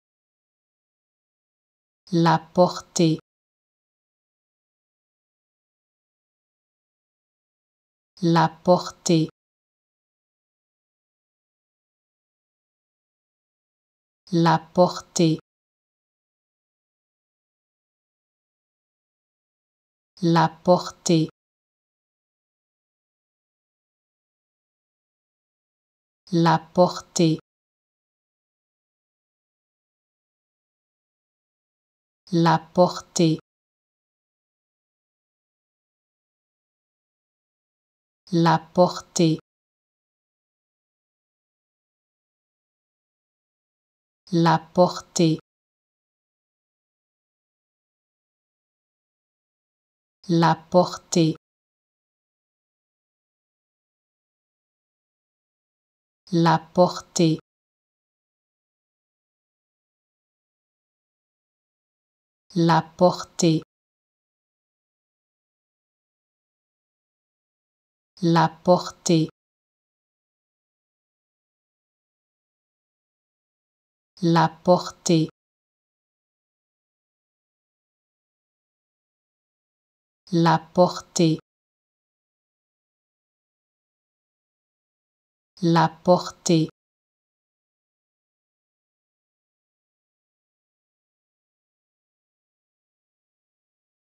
How to Pronounce ''La portée'' (The scope) Correctly in French
Learn how to say and properly pronounce ''La portée'' in French with this free pronunciation tutorial.